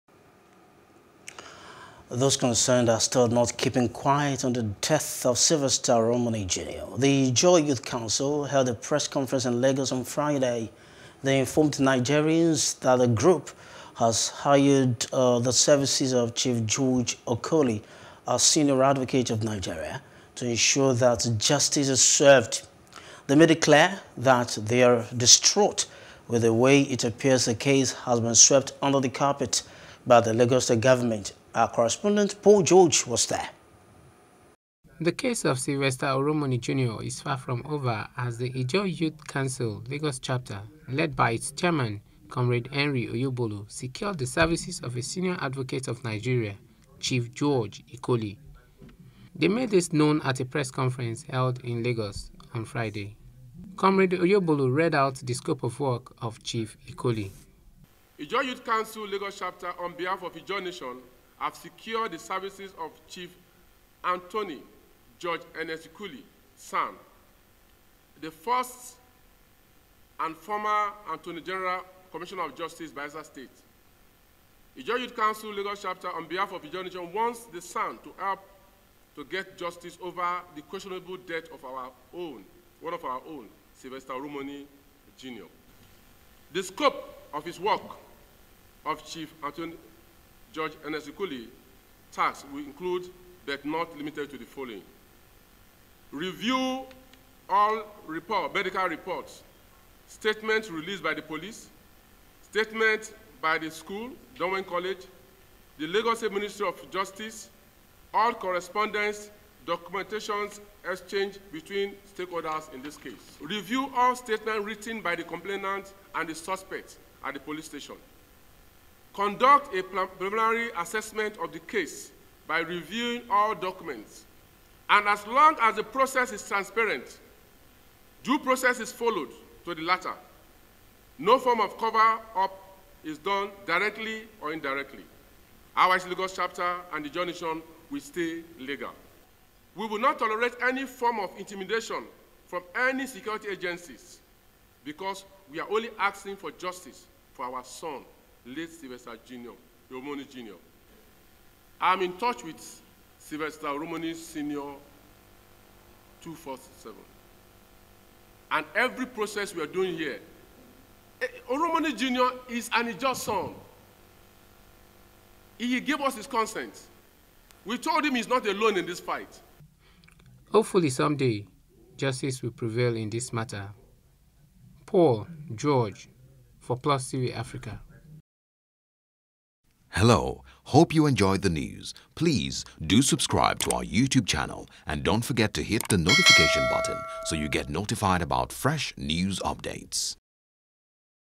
Sylvester Oromoni: Ijaw Youth Council Heads To Court For Justice | NEWS
Those concerned are still not keeping quiet on the death of Sylvester Oromoni Jnr. The Ijaw Youth Council held a Press Conference in Lagos on Friday.